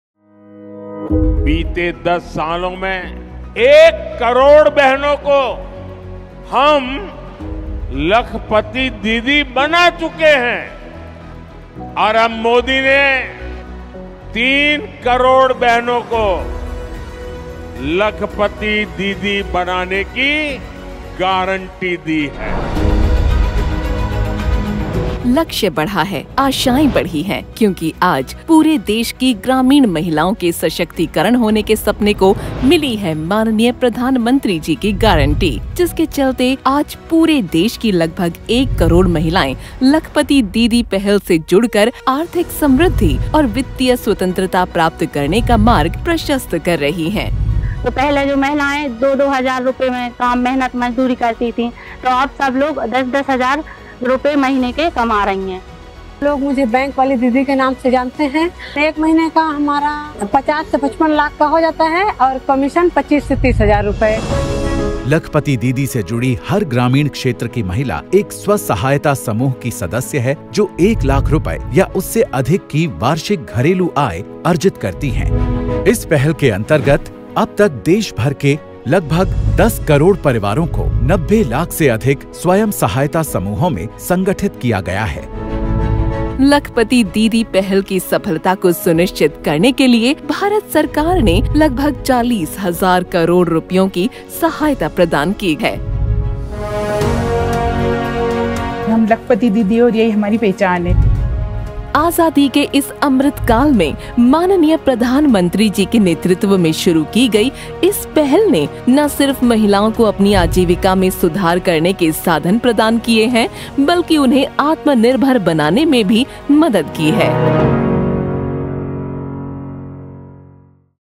Making '3 crore Lakhpati Didis' is PM Modi's guarantee
Under the able administration of Prime Minister Narendra Modi has truly empowered India's Nari Shakti. Their financial empowerment is a result of 1 crore Lakhpati Didis in India with Modi's guarantee of enabling 3 crore women become 'Lakhpati Didis'.